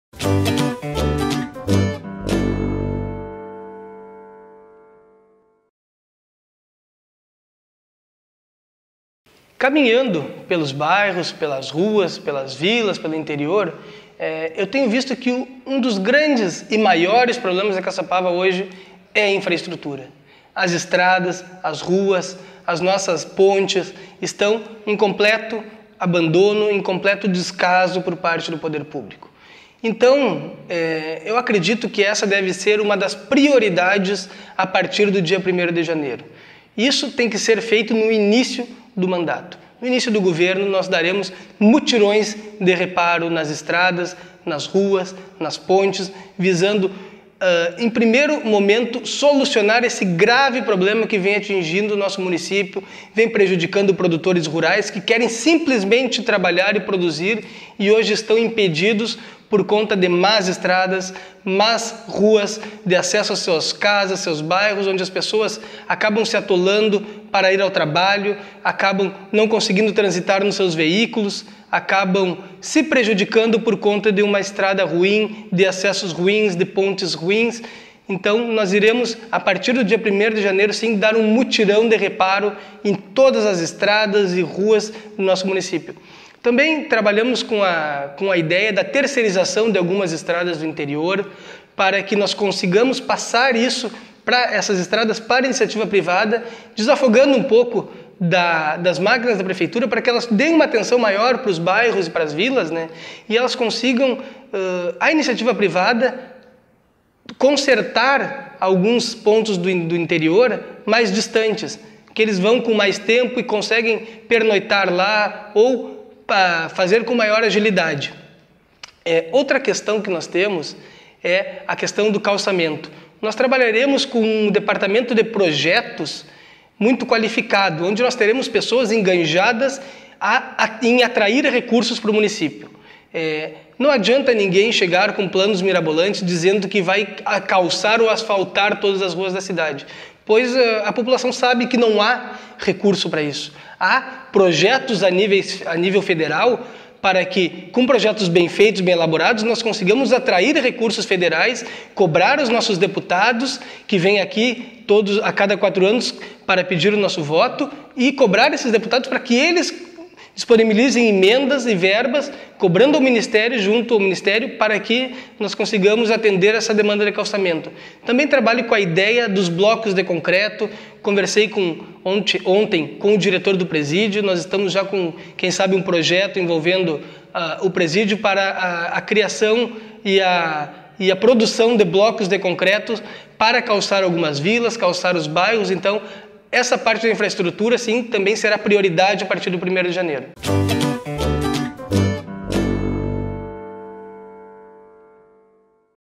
Eleições 2016: Projetos para Infraestrutura - Giovani Amestoy
O Farrapo preparou uma série de entrevistas com os candidatos a Prefeito de Caçapava do Sul, abordando seis temas: Saúde, Educação, Infraestrutura, Agronegócio, porque quer ser Prefeito e o conhecimento sobre a parte financeira da Prefeitura.